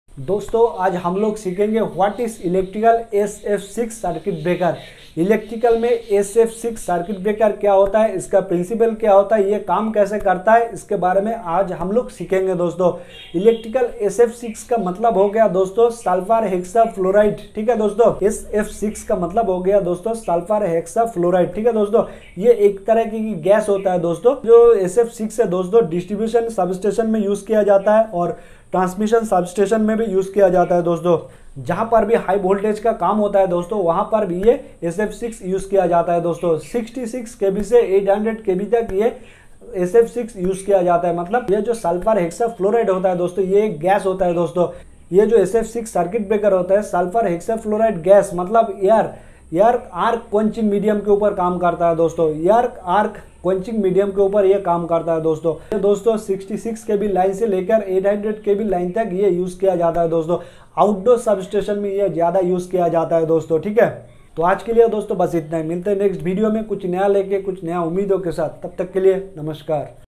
What Is Electrical SF6 Circuit Breaker ? | Electrical SF6 Circuit Breaker Kya Hota Hai ?
What Is Electrical SF6 Circuit Breaker ? || Electrical SF6 Circuit Breaker Kya Hota Hai ?? || This video briefing about what is electrical SF6 Circuit breaker.